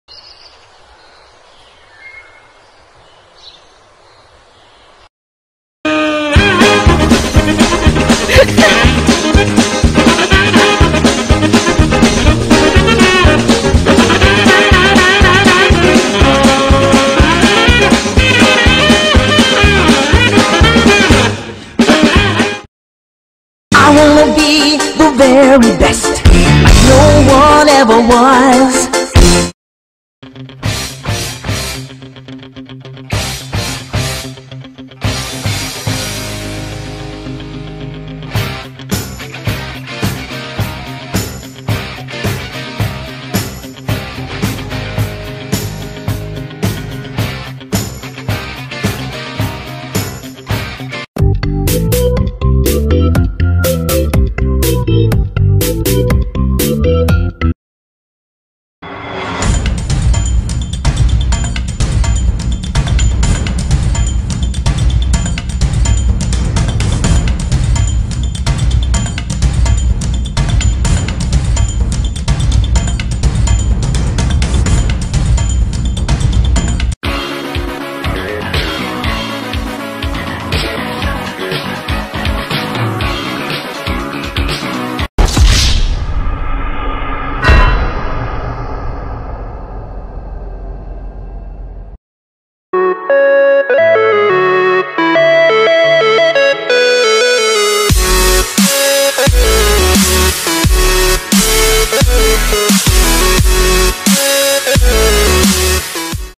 Suhaib's Dream - A short Comedy Montage [HD]
Suhaibs Official Inspirational Movie LELEL

Plot : In a distant past... Suhaib tries to climb a ledge. After failing miserably many times , he decides to push to the limits. He soon manages to climb over the ledge but he is met with an unexpected fate.